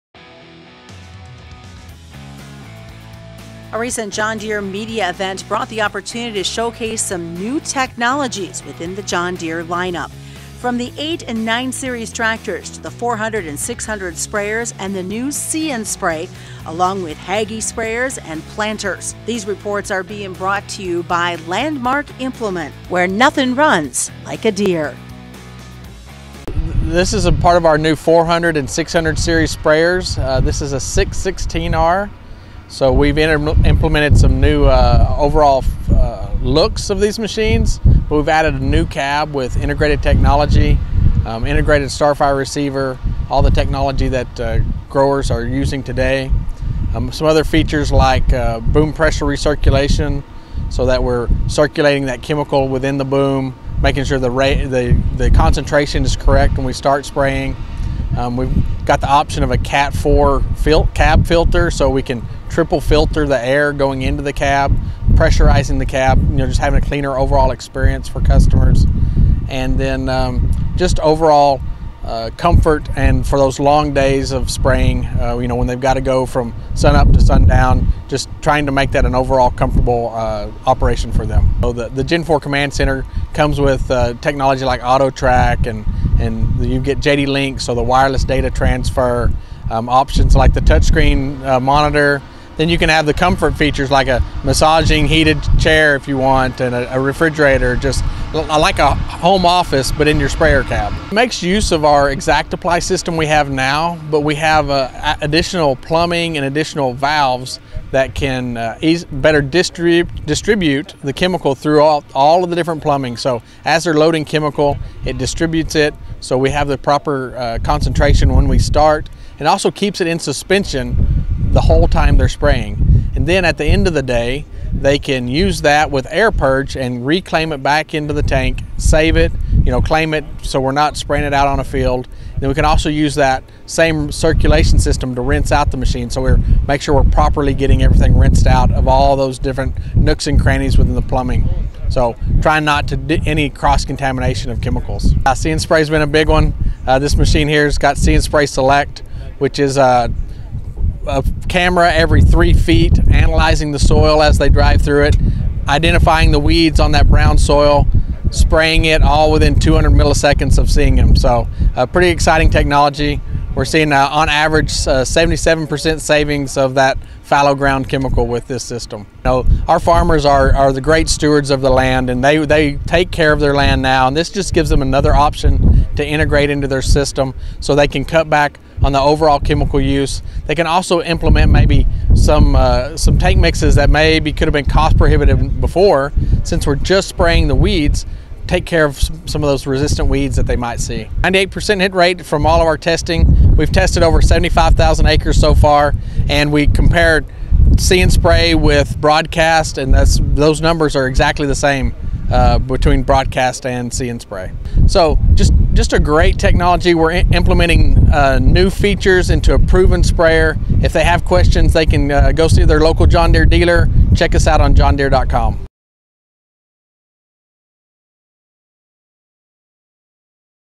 JD Sprayer with See and Spray Joel Basinger
Joel Basinger is the Application Marketing Manager for John Deere.  He talks about See and Spray along with new technology updates